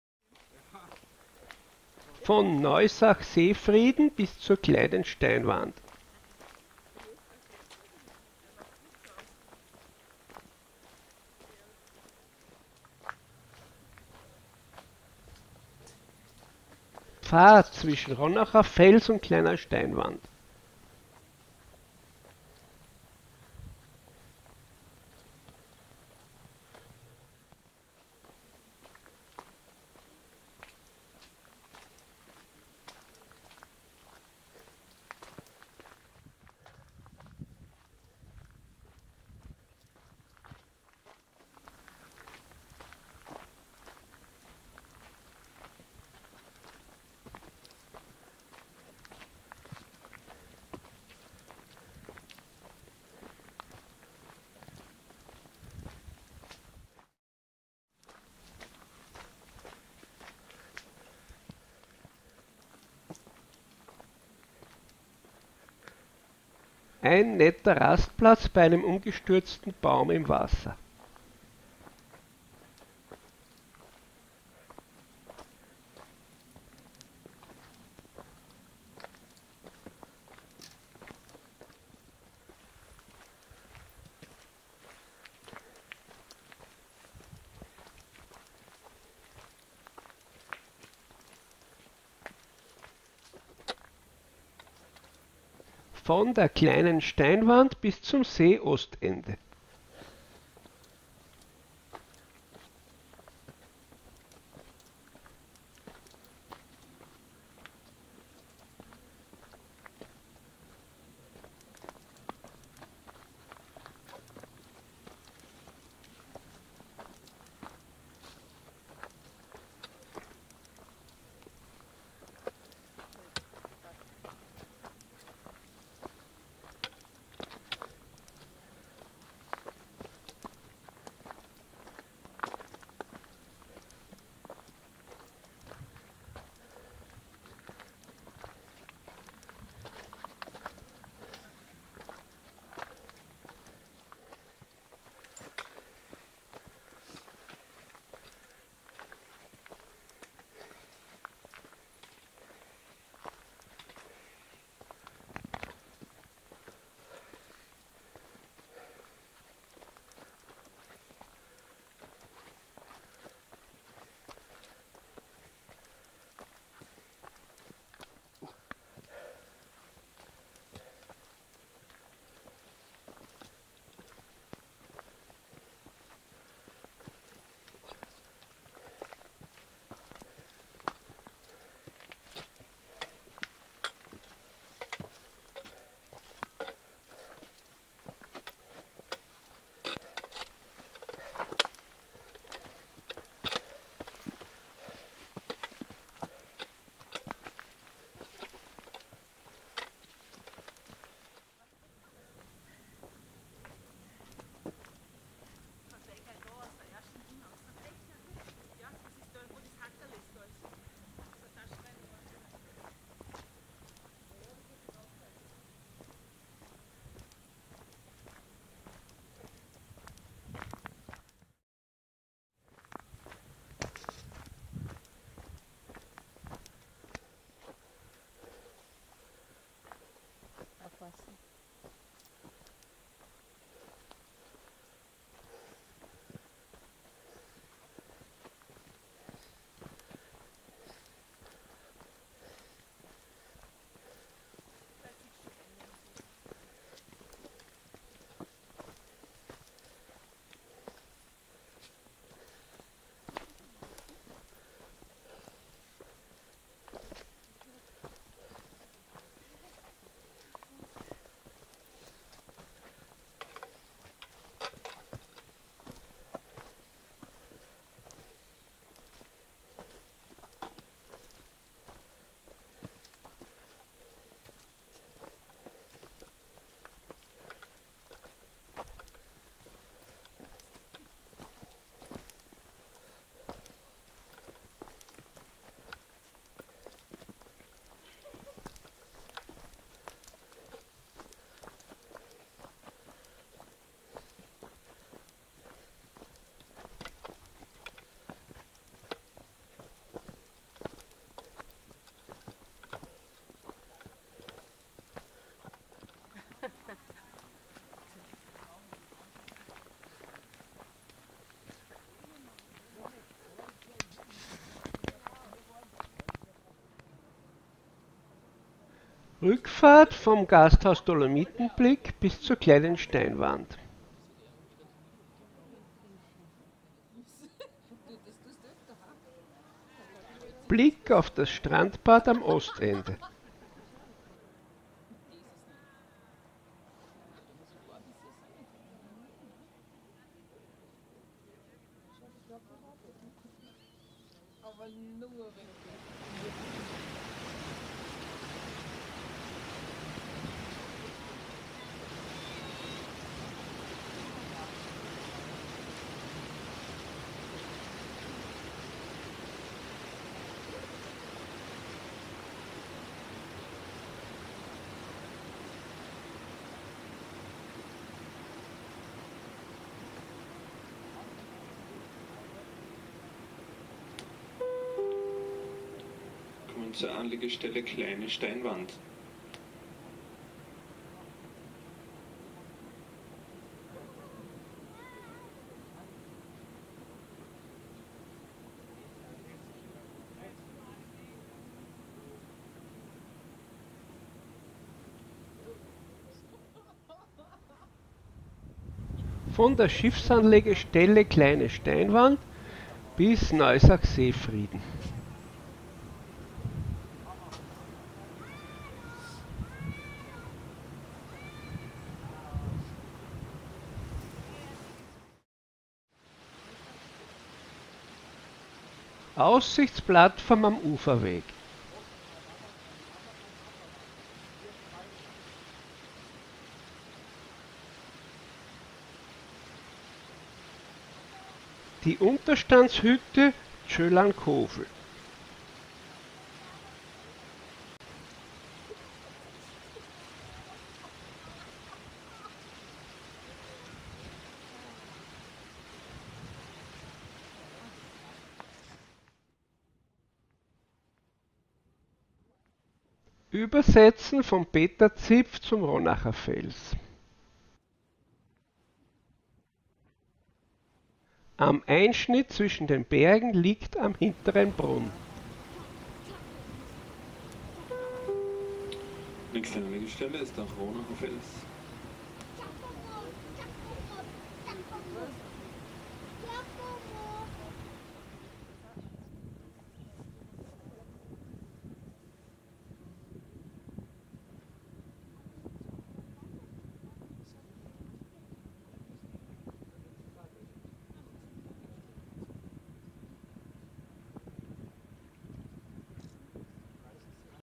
Weissensee Steinwandpfad
360°-Video meiner Wanderung "Weissensee Steinwandpfad" auf bergfex.at
0:00 Von Neusach Seefrieden bis zur Kleinen Steinwand
1:34 Steinwandpfad (von Kl. Steinwand bis Ostende)
5:17 Rückfahrt mit Schiff bis Kl. Steinwand
6:37 Weitere Rückfahrt bis Ronacherfels